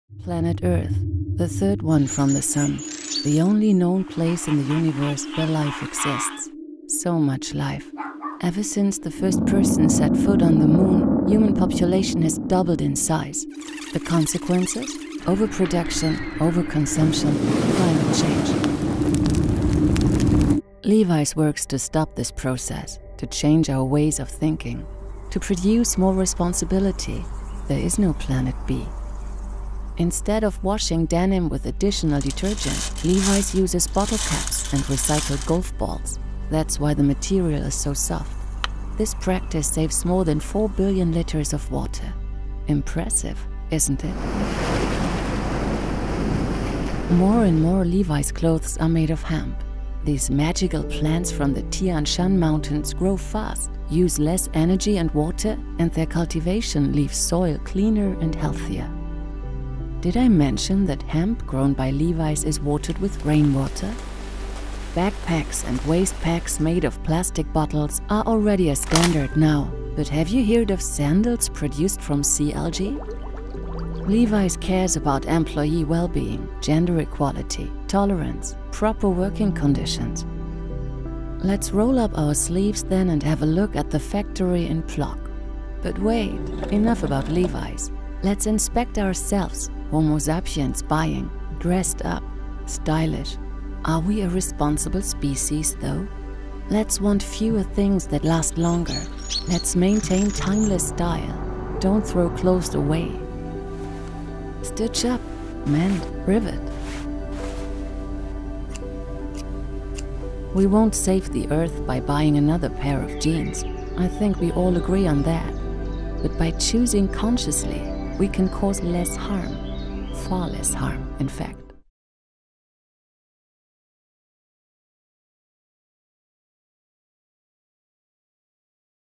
LEVI'S Digital 2021 Frankfurt - Fashion Channel
Jeans InnovatorLevi's will show the compatibility of sustainability, diversity and fashion with an inspiring and artistically staged film.

Levi’s, the signature brand of the clothing company Levi Strauss & Co., is famous for creating and popularizing the blue jeans that have become a staple in every American wardrobe.